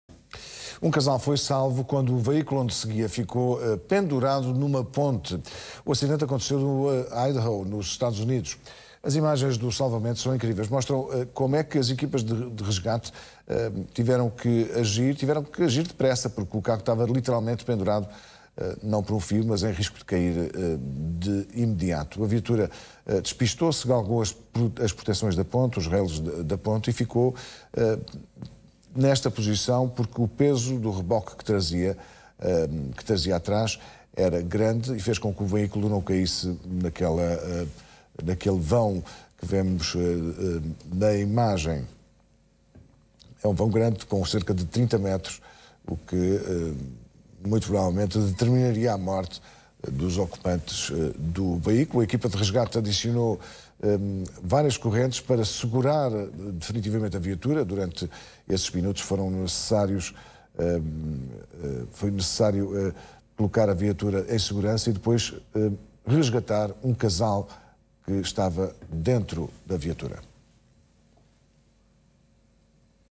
CASAL É SALVO COM O CARRO PENDURADO EM PONTE!
Um casal nos estados unidos foi salvo depois do carro ter ficado preso numa ponte, o carro estava prestes a cair!